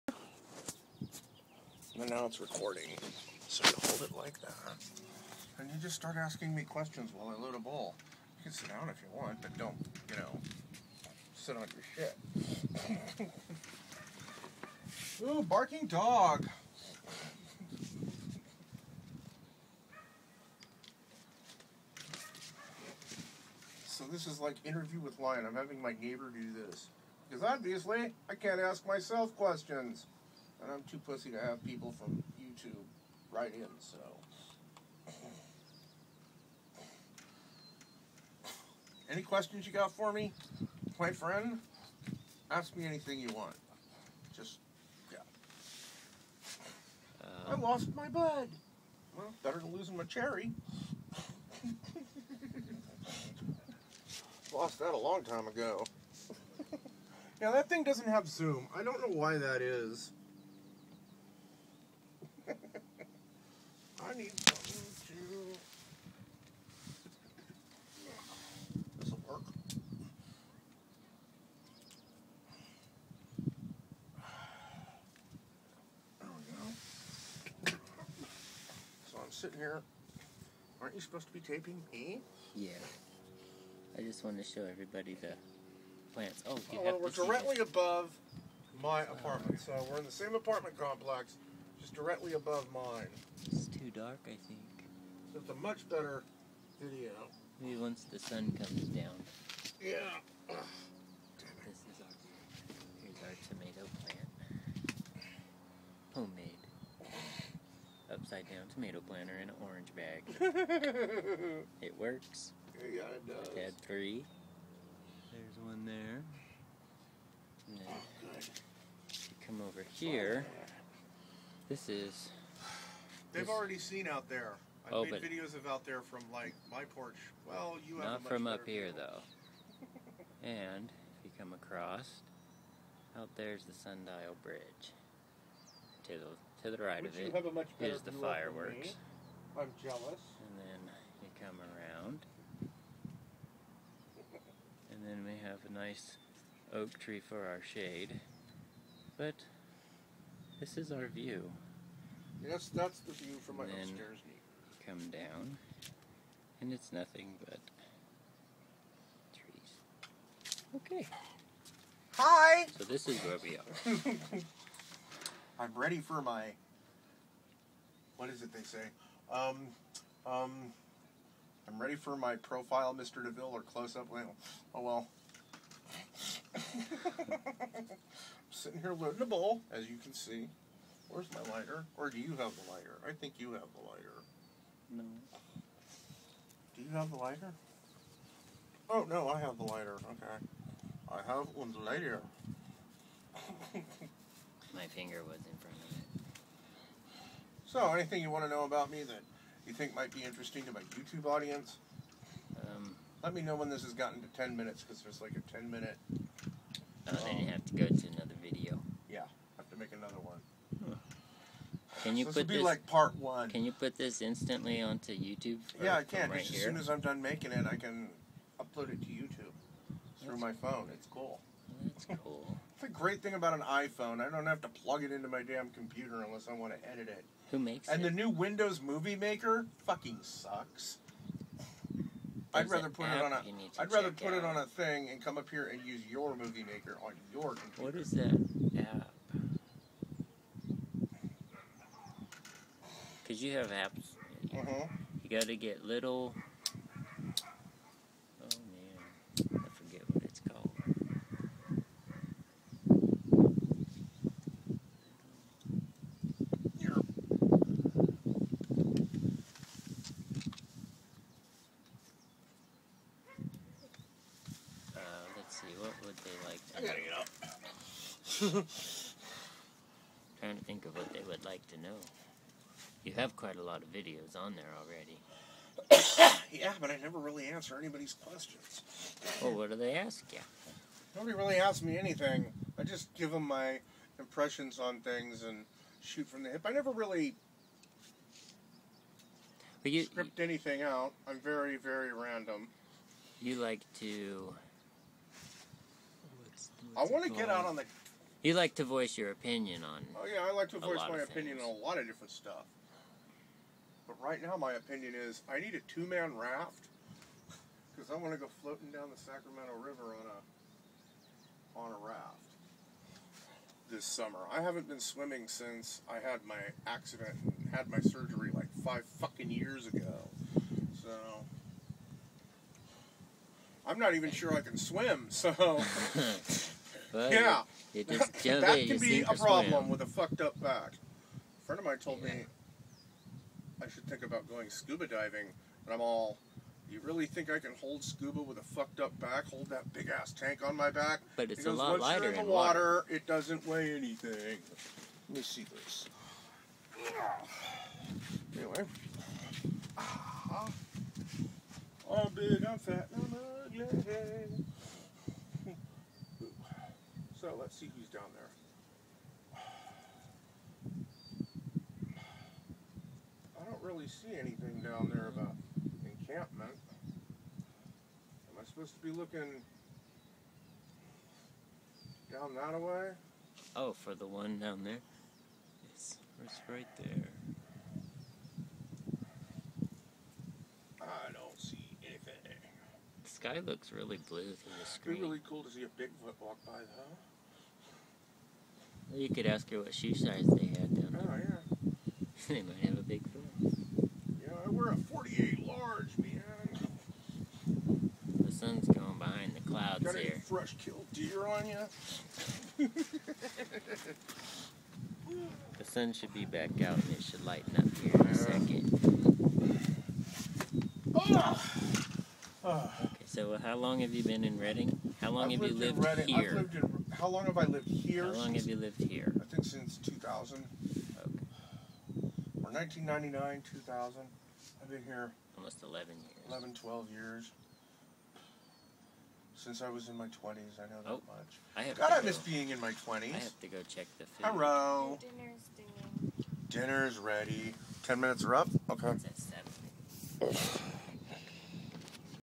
Hanging with my neighbor pt. 1
Me at my upstairs neighbors balcony hanging out.